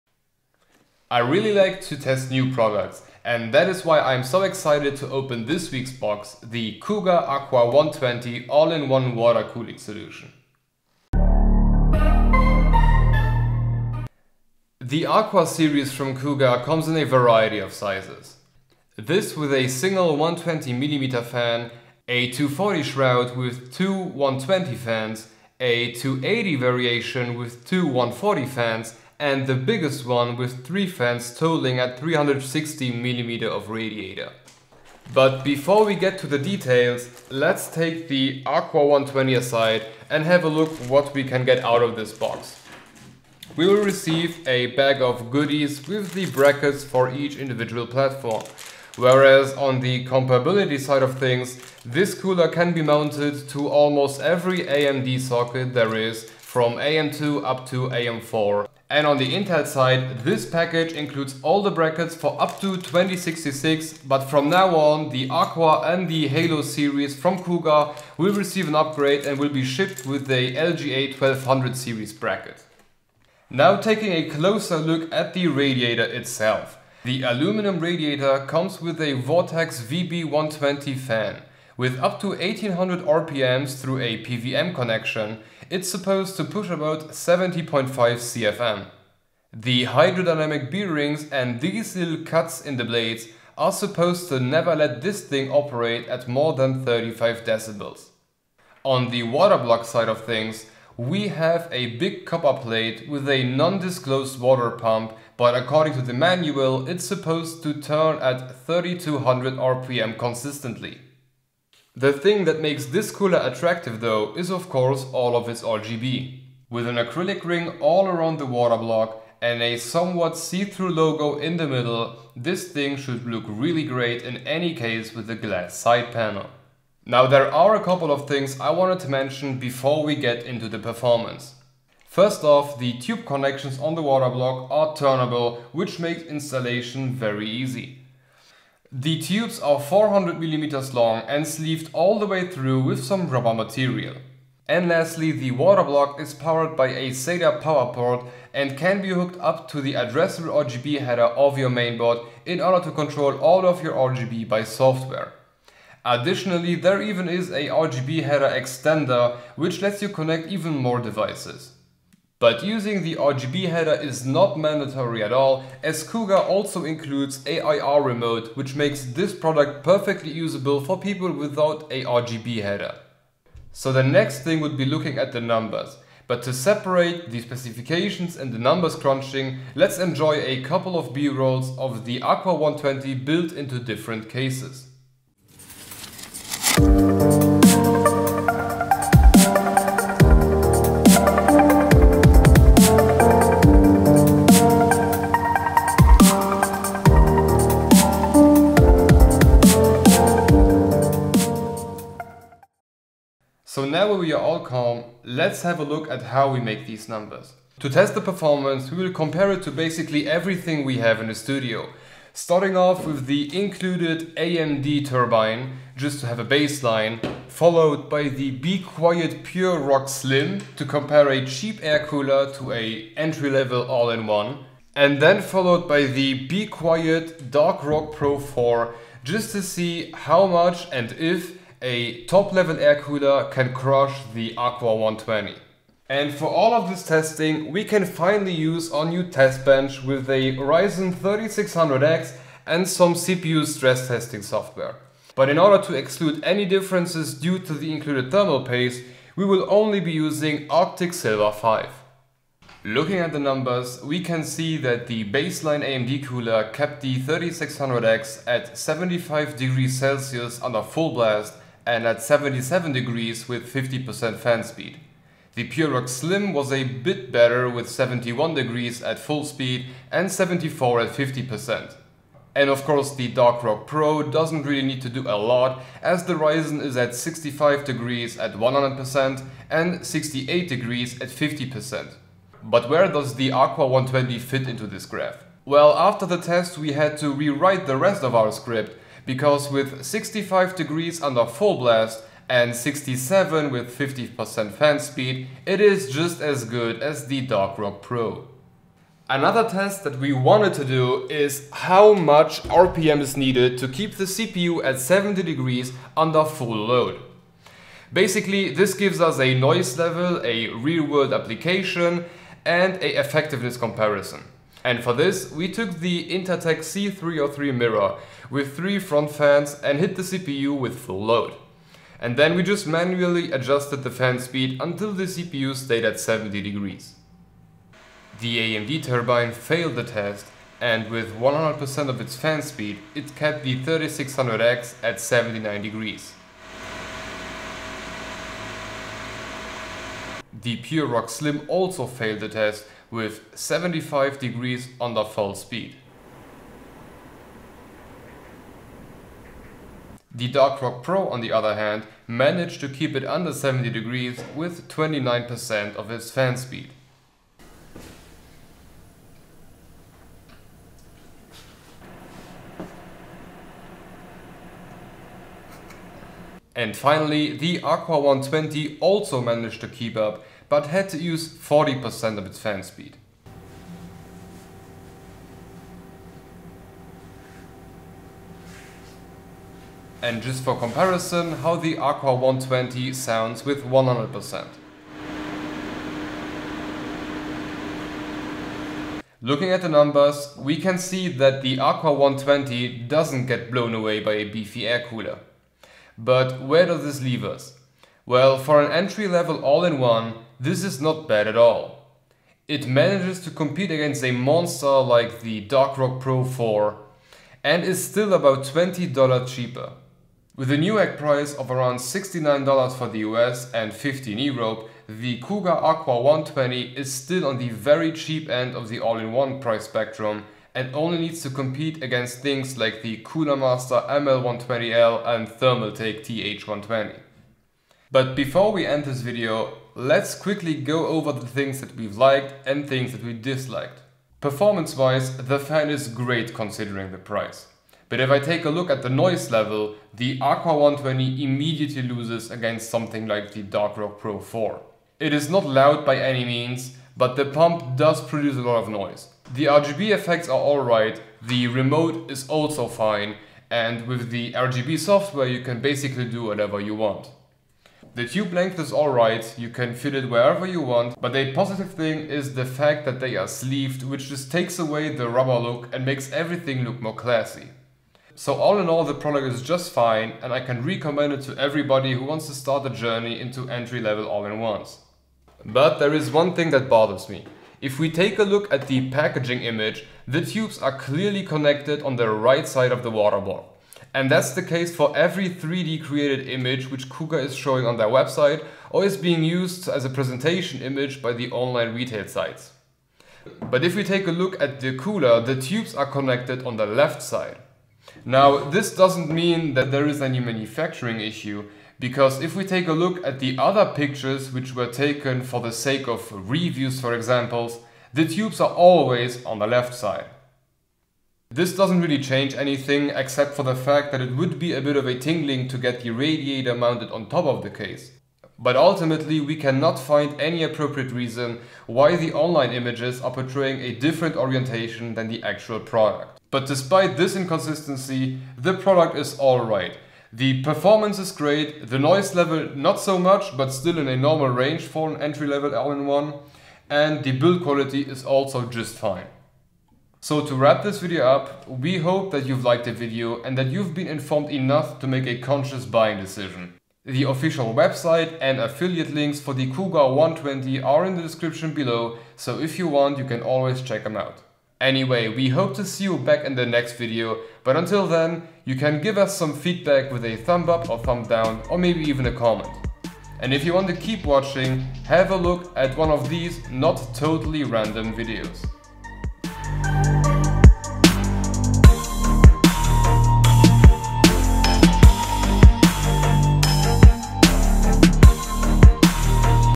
Cougar Aqua 120 RGB | Watercooling Review
The Cougar Aqua 120 RGB AIO Watercooler offers an entry-level Watercooling experience for an entry-level price, let's have a look at it and compare it to established Air Coolers like the be Quiet! Dark Rock Pro 4!